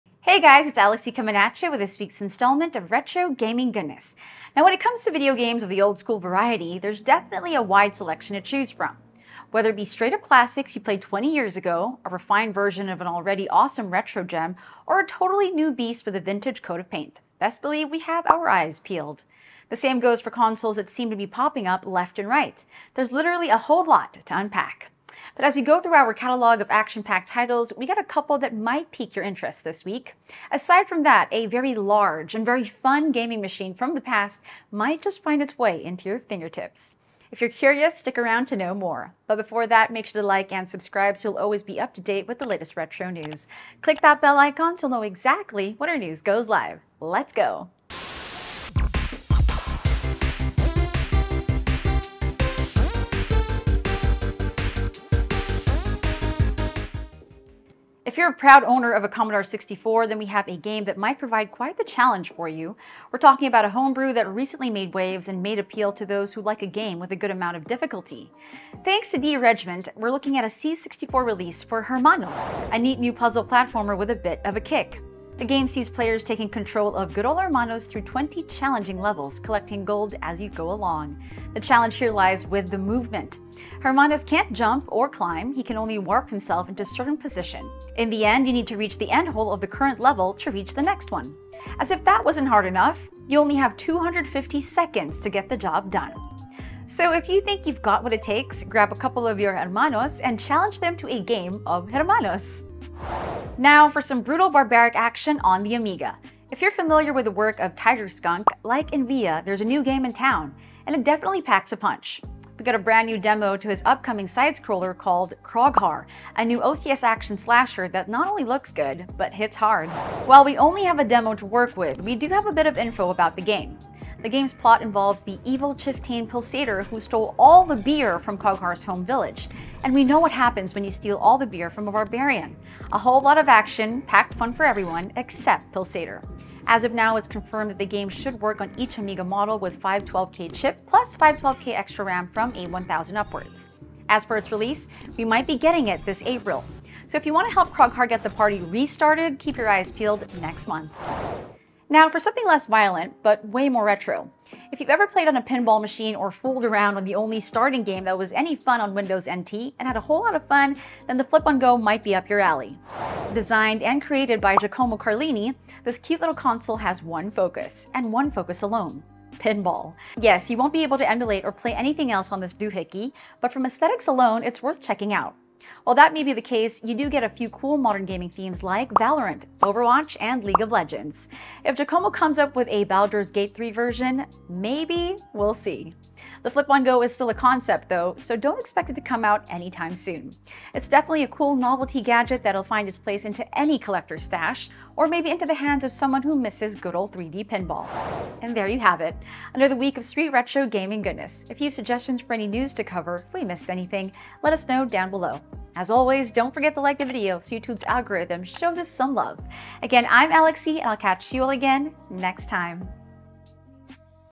Mega Cat Studios Dose of Gaming News: Hermanos, Krogharr, and FlipOnGo
Welcome to the latest episode of Gaming News with Alex Sy! In this feature we're going to be checking, the homebrew, puzzle, platformer, Hermanos by D.Rejiment for the Commodore 64; the action side scroller for the Amiga, Krogharr, by Tigerskunk; and a new concept for a console focused on Pinball, FlipOnGo by Giacomo Carlini!

Explore the featured titles of the week below, and let's dive headfirst into the excitement of gaming!

0:00 New Titles

0:58 Hermanos:

1:43 Krogharr:

2:31 FlipOnGo:

3:18 See you on the next gaming news!